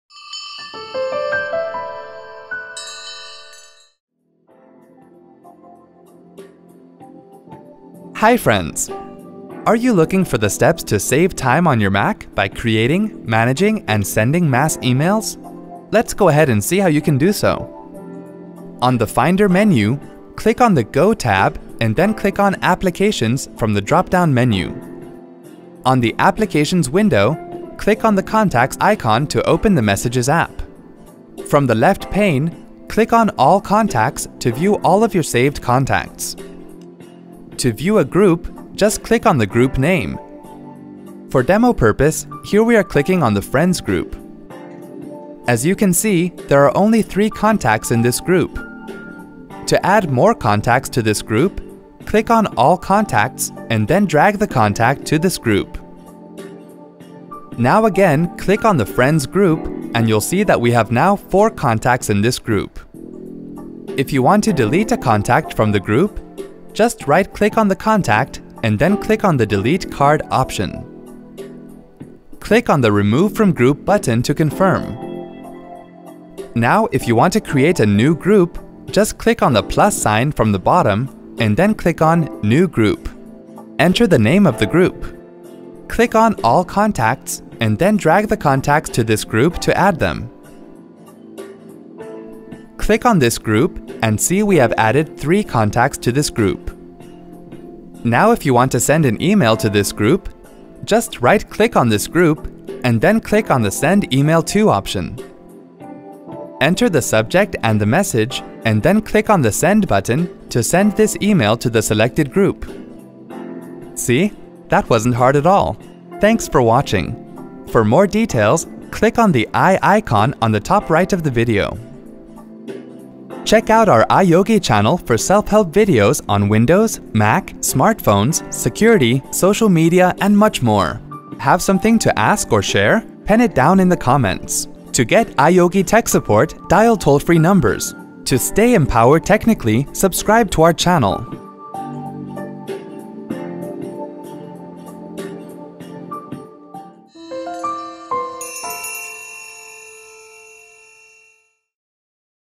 How to create, manage and send group emails on a Mac® :Tutorial
Are you looking for the steps to save time on your Mac® by creating, managing and sending mass emails? Get to know about easy steps to create, manage and send group e-mails on a Mac® by watching this video.